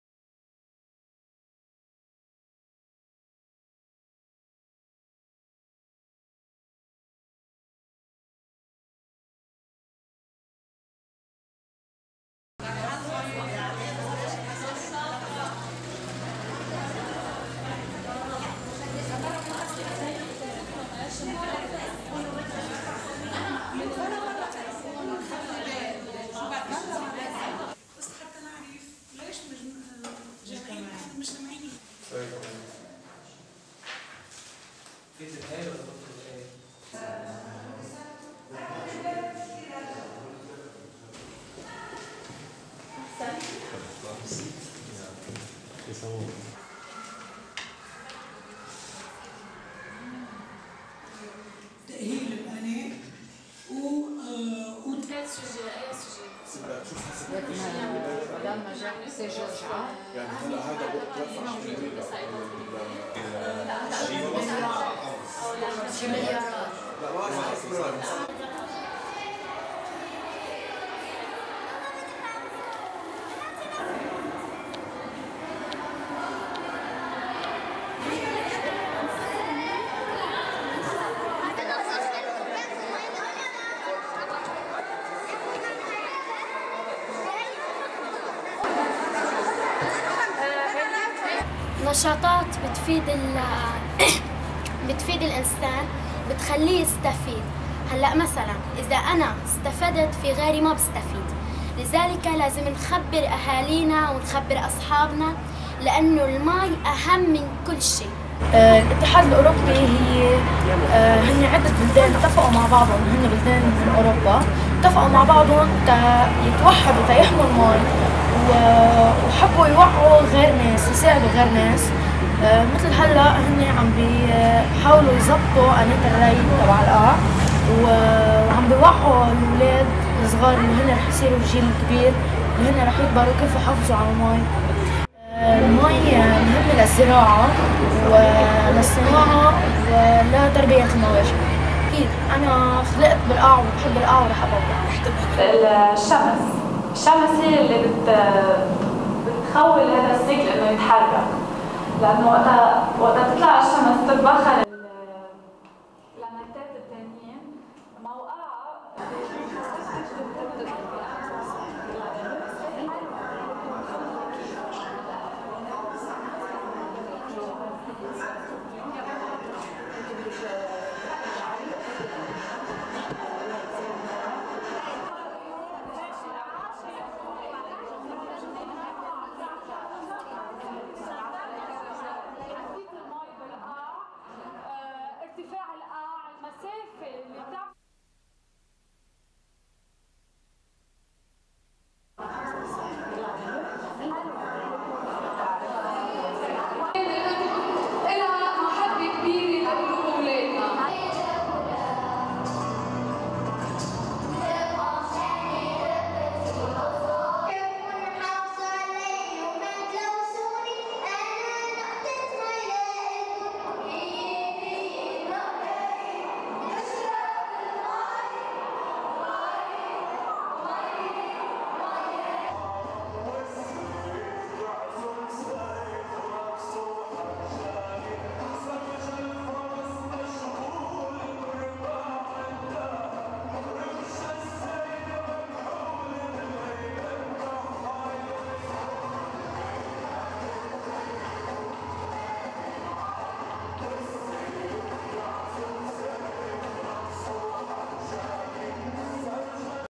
EU supports water awareness activities in the Qaa
Baseline knowledge assessment and awareness activities about water use and conservation in households organised under EU-funded project in the Qaa region